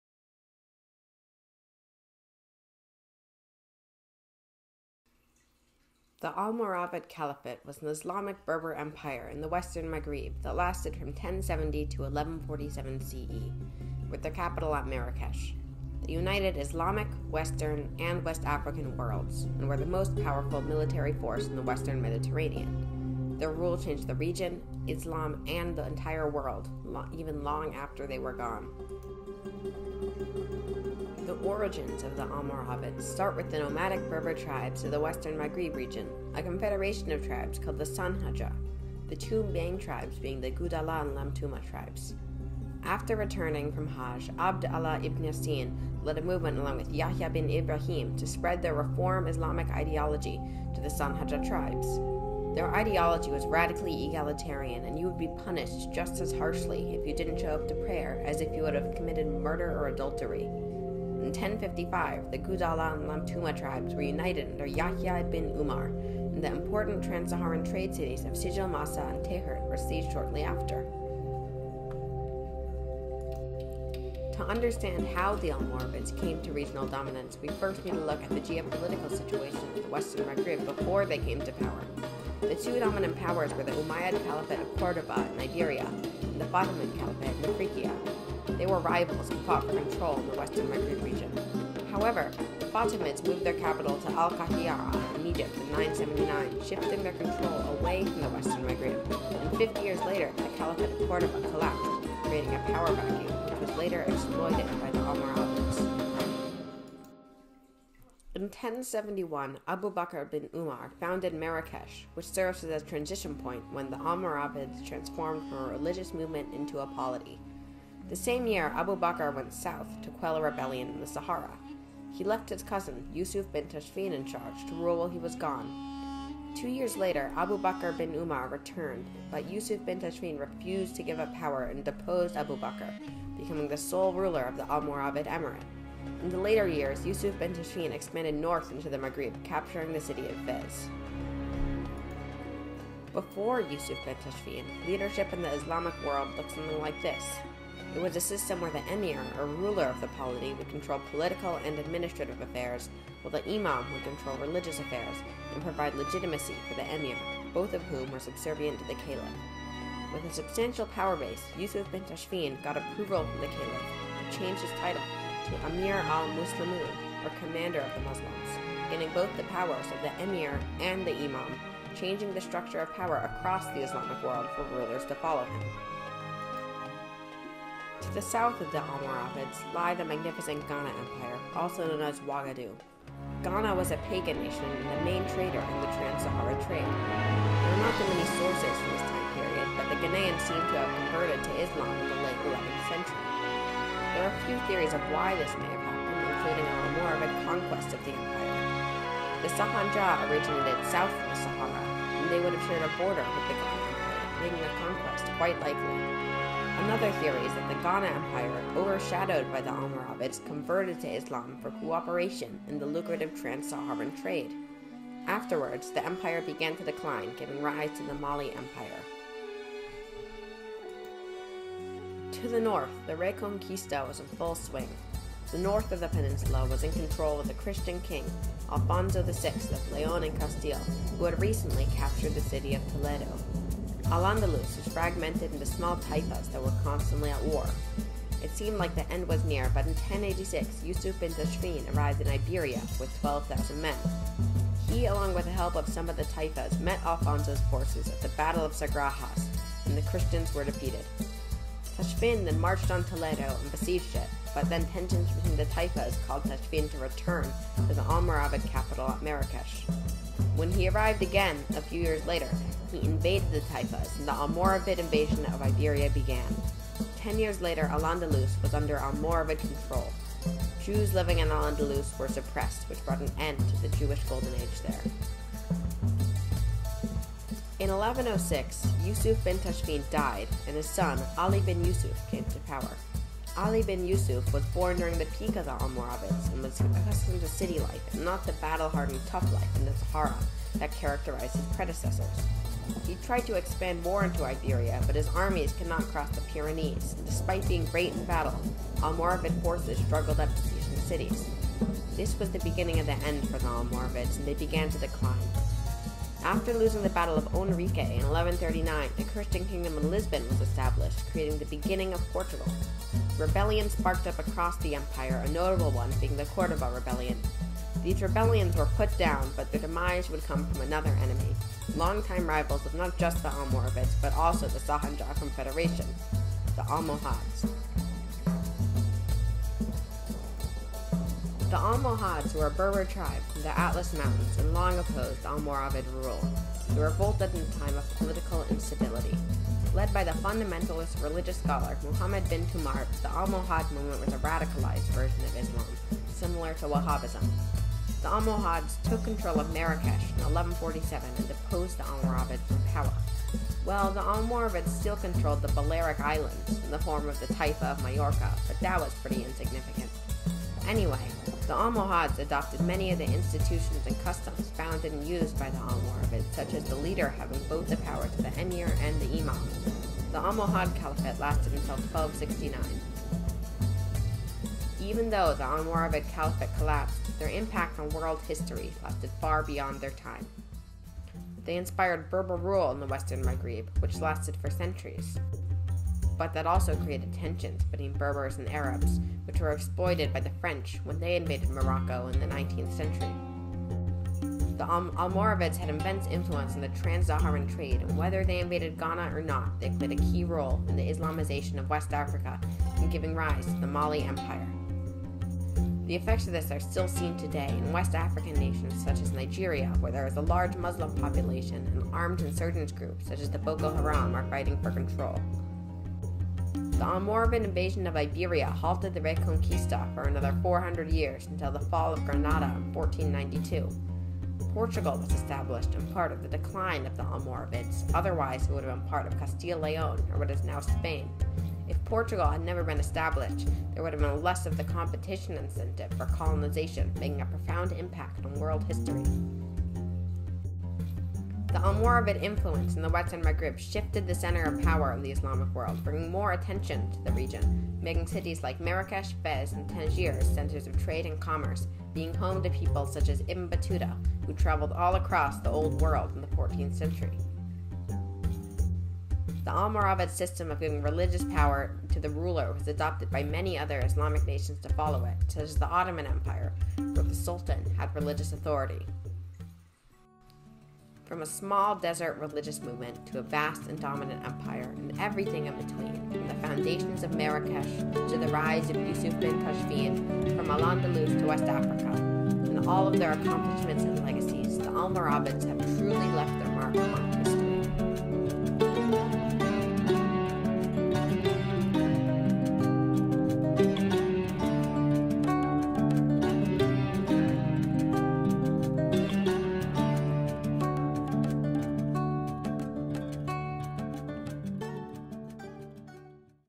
The Almoravid Caliphate (1070-1147 CE)
The Almoravid Caliphate was an islamic berber empire in the Western Maghrib. In this video, I will discuss the fascinating history of the Almoravid Caliphate and their profound impacts that they made on the world, to this day.